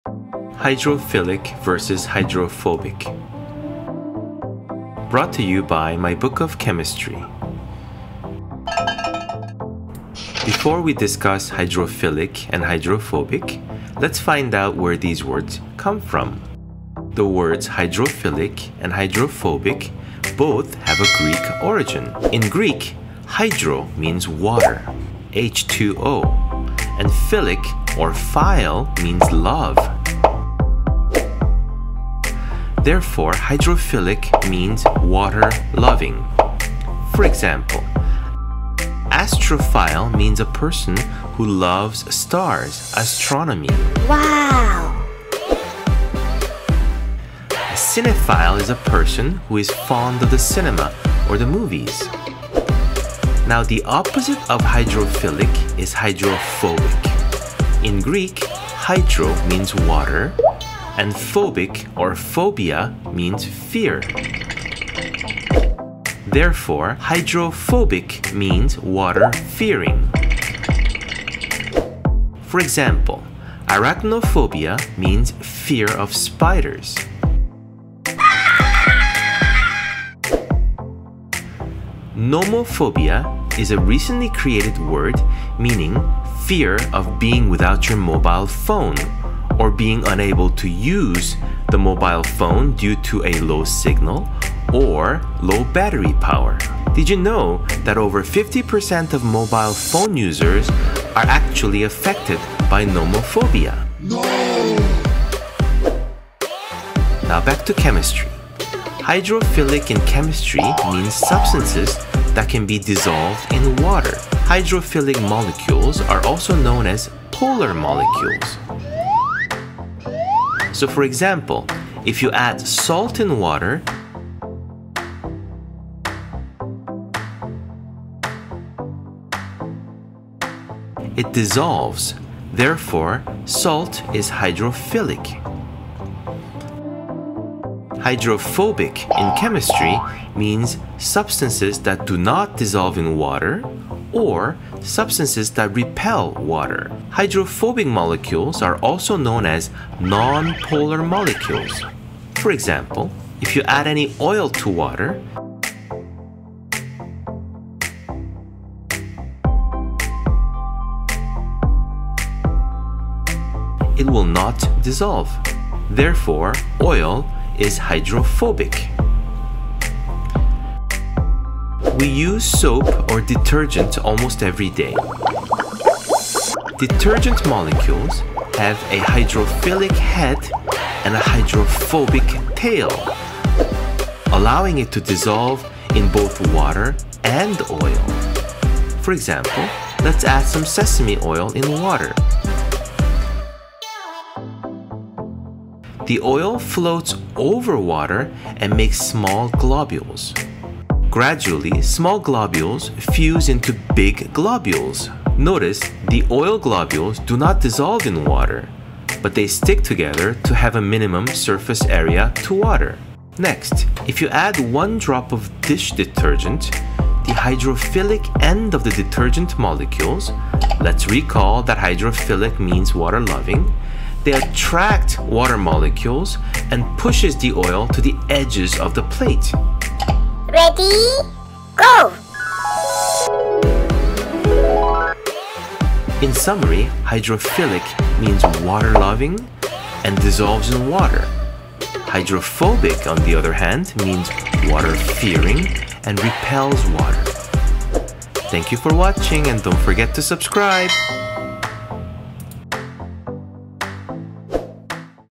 Hydrophilic vs Hydrophobic
What does 'hydrophilic' mean? What does 'hydrophobic' mean? This video explains them with fun animation and examples.